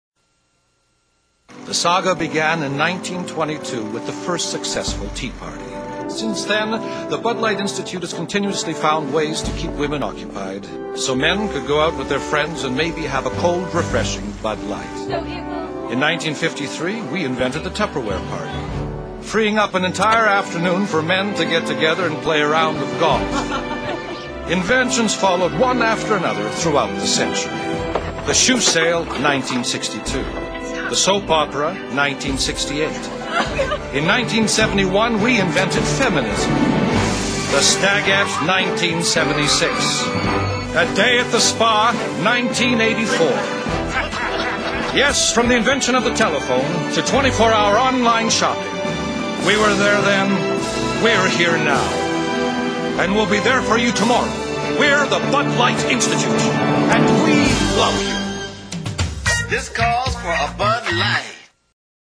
Beer Institute's Inventions for Women
How to keep Women occupied so the men can do 'Guy' things.